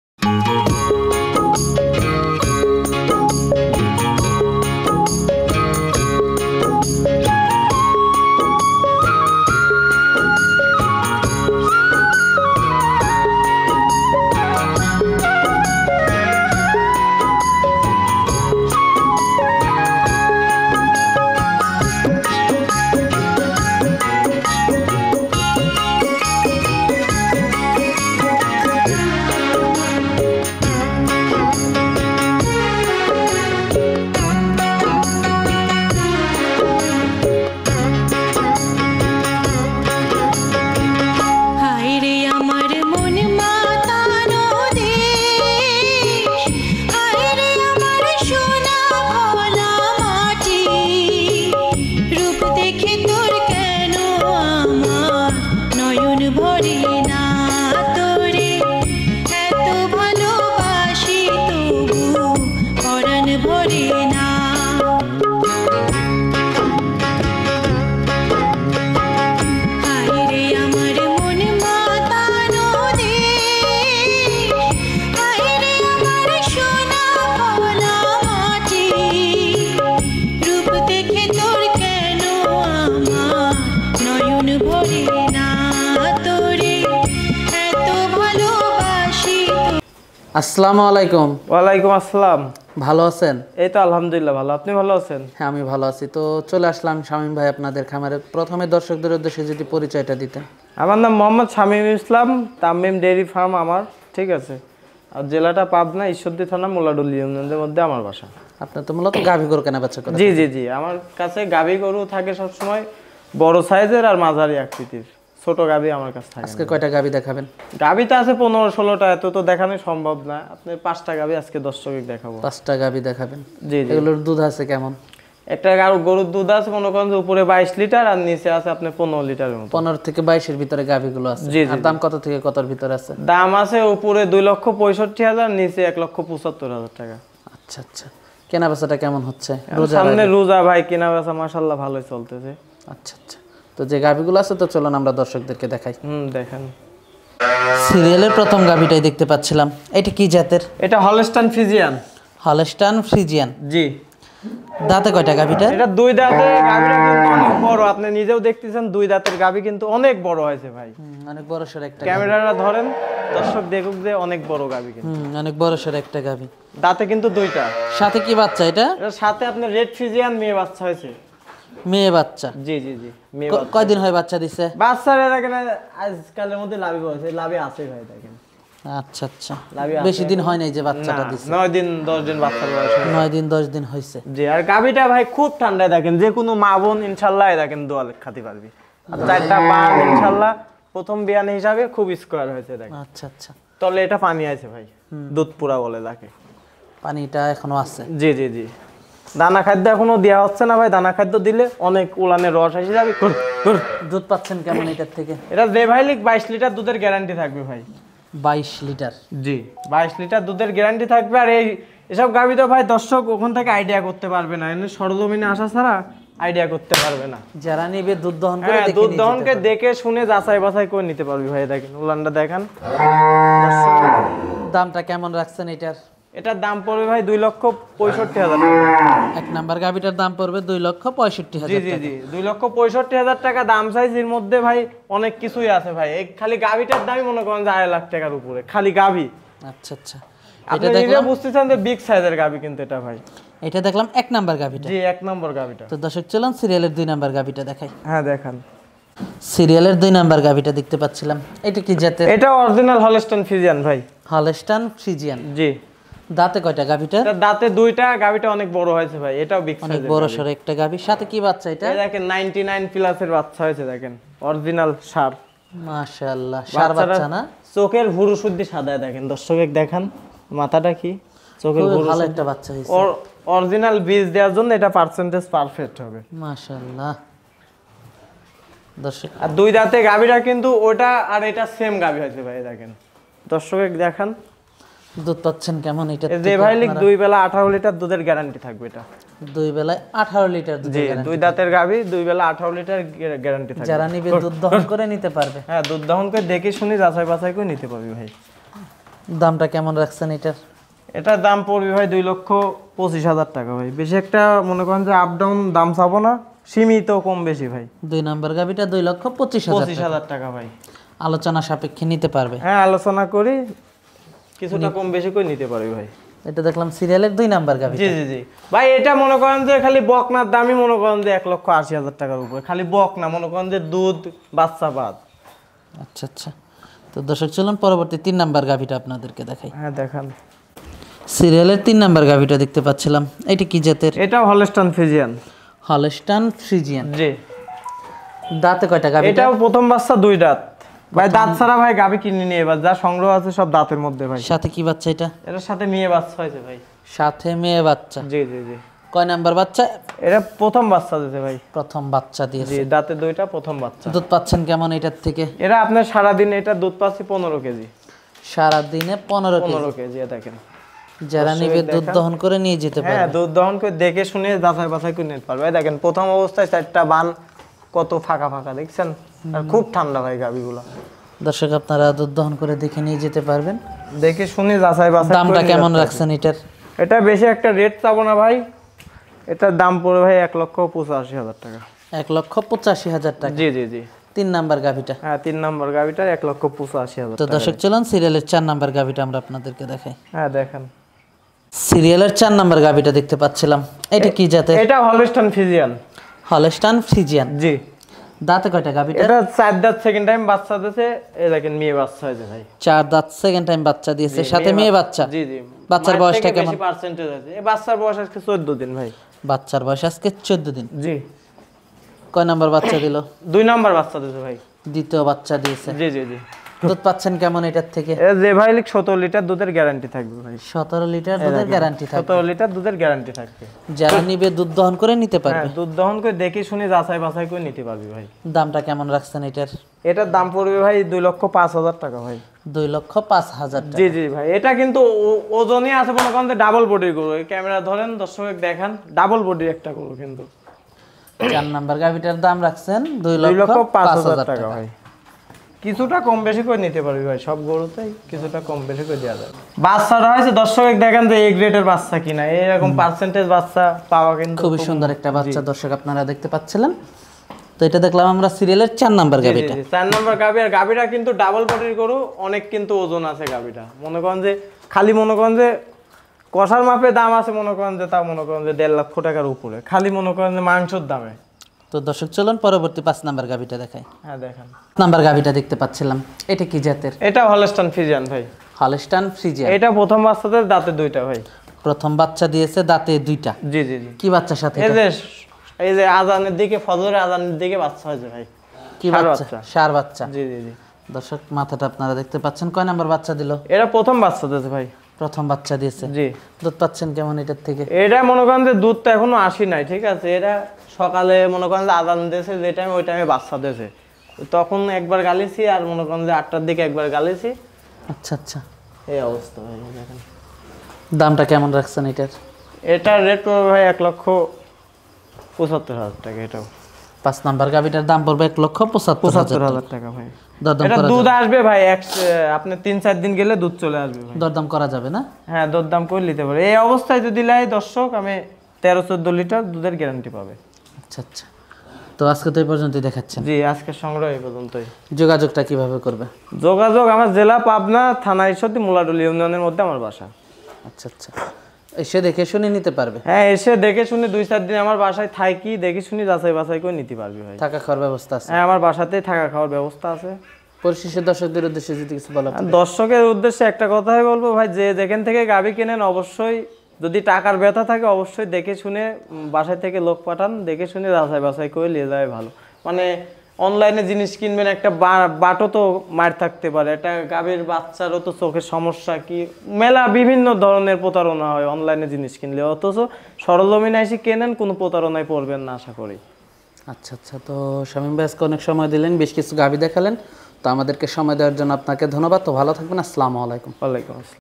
এ বছরের সেরা প্রতিবেদন। গাভী প্রতি ১০-২০ হাজার টাকা কম। ক্রেতারা সরাসরি গাভীর দুধ দোহন করে নিন
বিঃদ্রঃ ( প্রতারনা এড়াতে) যে যে স্থান থেকেই গরু কিনুননা কেন অবশ্যই নিজে দেখে দরদাম করে নিজ দায়িত্বে কিনবেন ।

আপনাদের যে কোন মতামত পরামর্শ কমেন্টে জানাতে পারেন। নতুন আপডেট পেতে আমাদের চ্যানেলটি সাবস্কাইব করে রাখুন। ধন্যবাদ